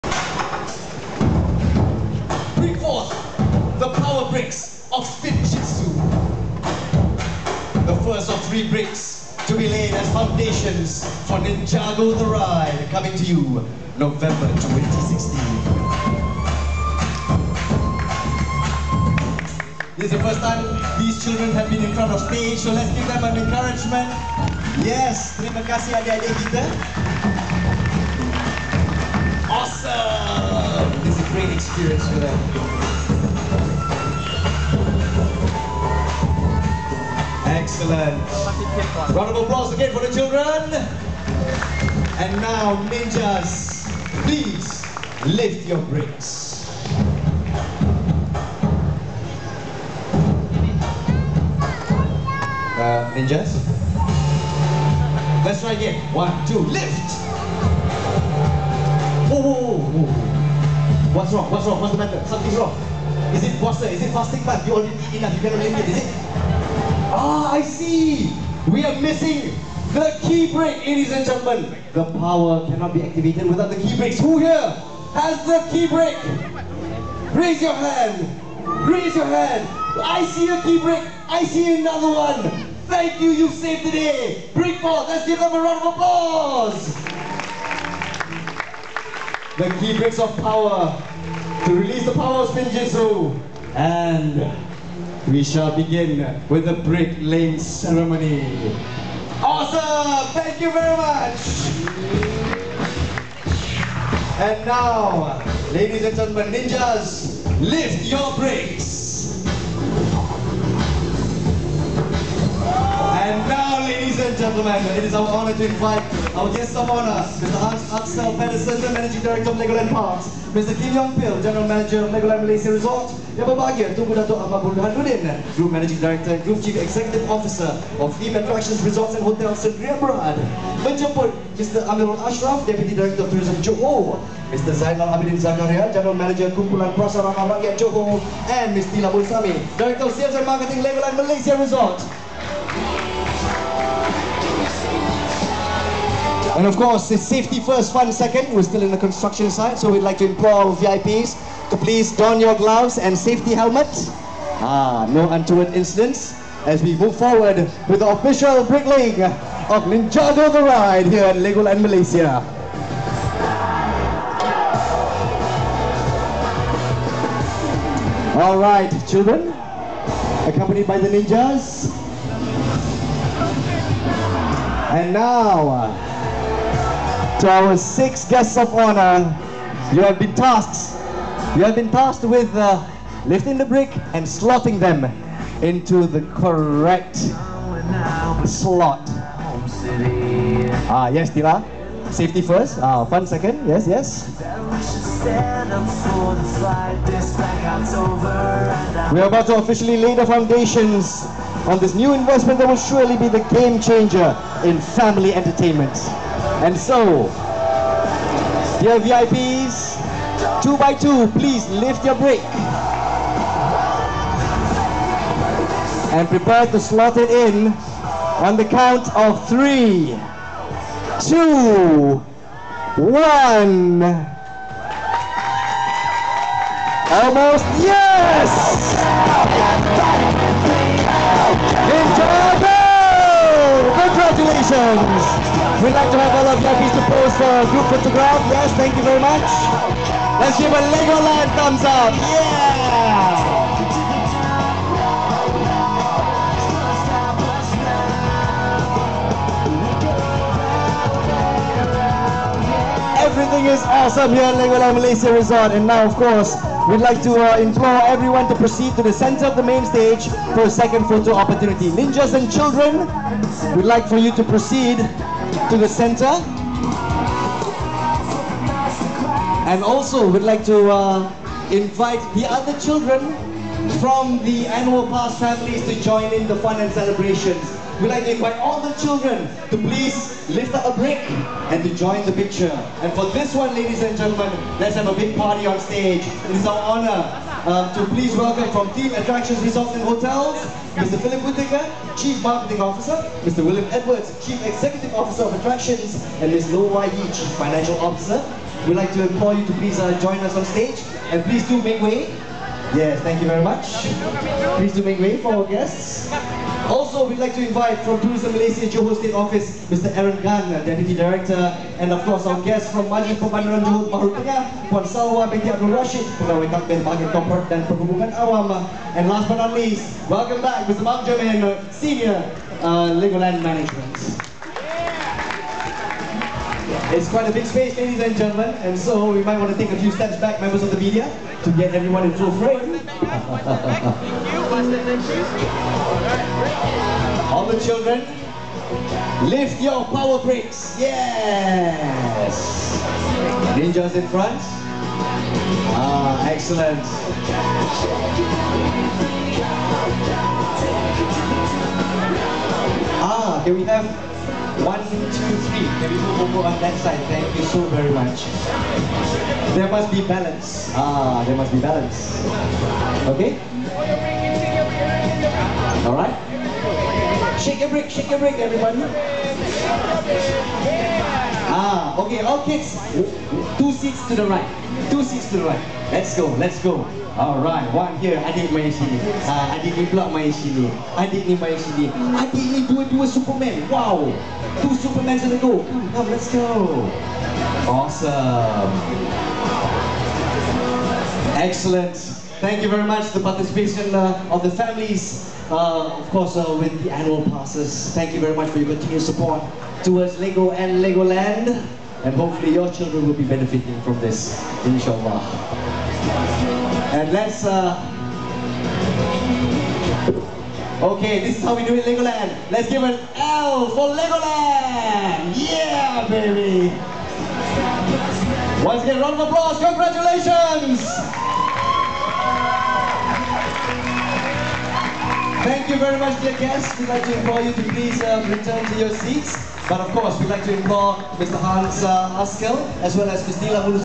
LEGOLAND Malaysia Resort Unveils LEGO NINJAGO The Ride-Lego Brick-Laying Ceremony
LEGOLAND Malaysia Resort unveils LEGO NINJAGO "The Ride" expected to be open in November 2016.